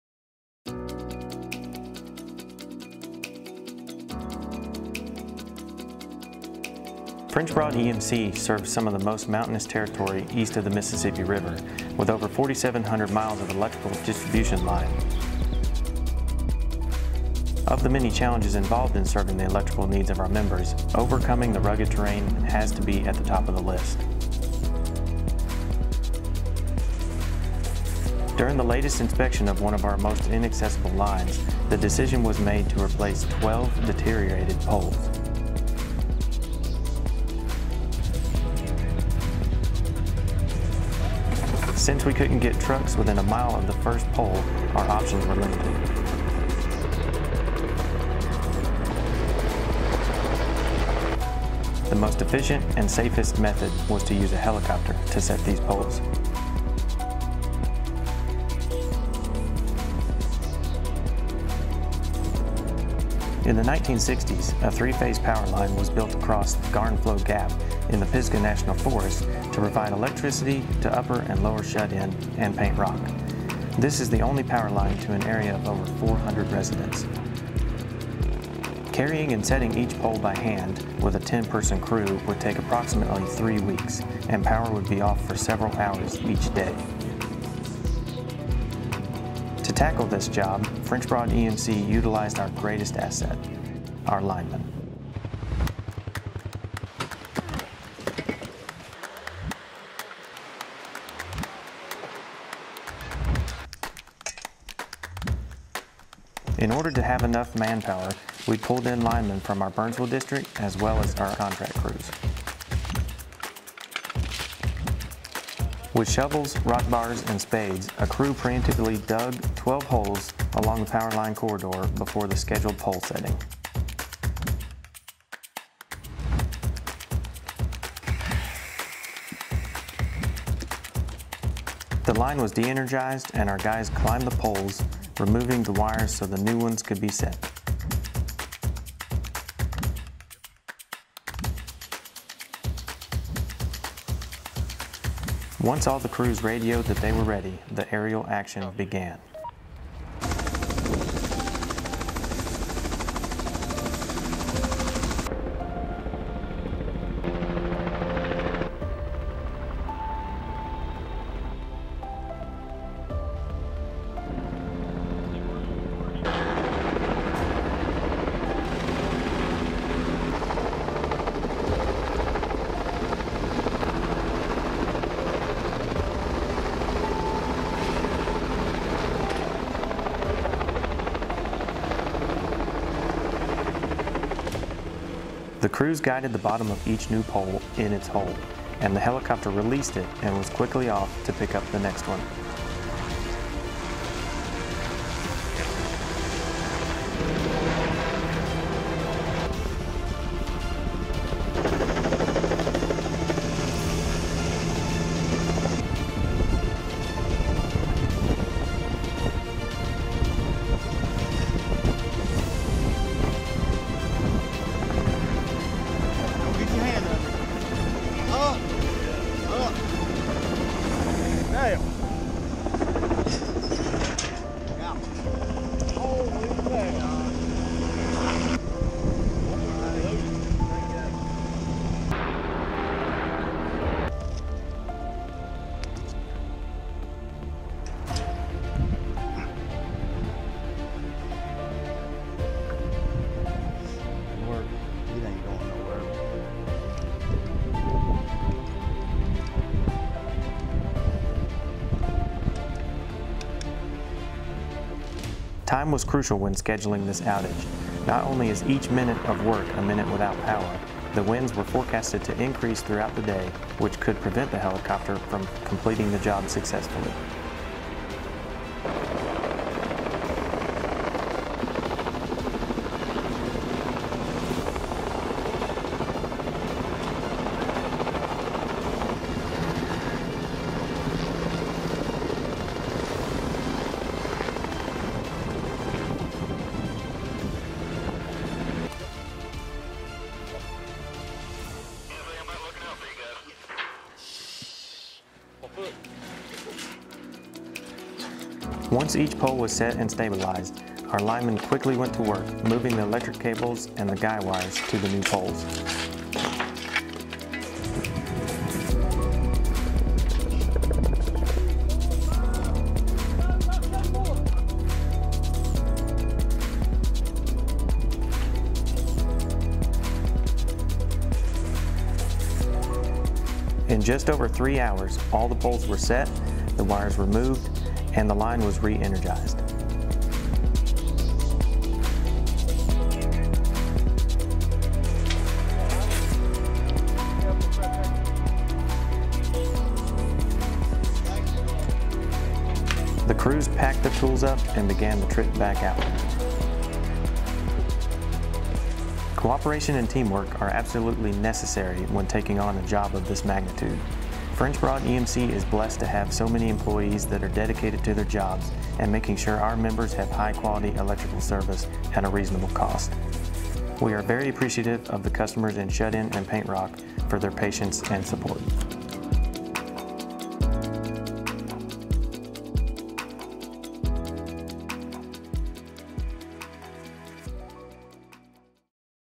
Helicopter Pole Setting
Ensuring consistent electric service to FBEMC members in remote areas of our service territory presents some unique challenges. Check out what it looked like to replace twelve deteriorated poles in one of the most inaccessible stretches of electrical distribution line in WNC.